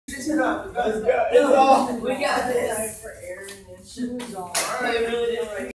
School Track Test
random footage from my phone, decided to motion track and these are the results